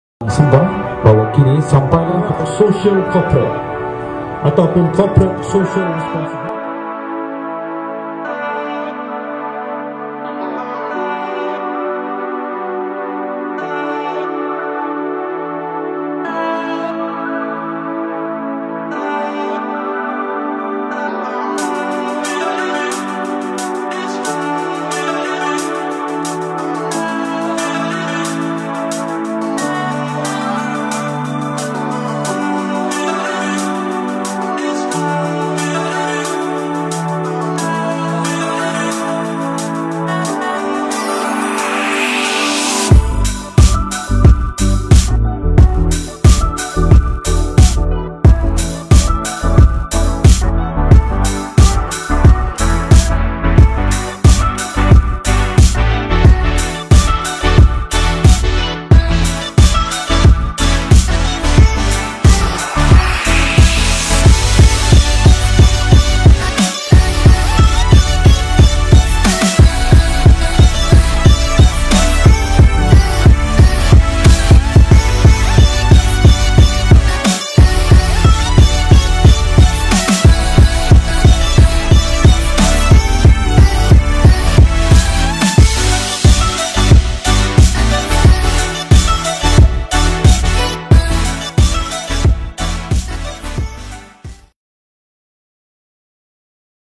Pusat Kecemerlangan Orang Asli Perak (PKPOAP) - 2020
Drone View Version 2 Sempena 25 Ulang Tahun YOAP / POAP. Capture with DJI Mavic Pro 2.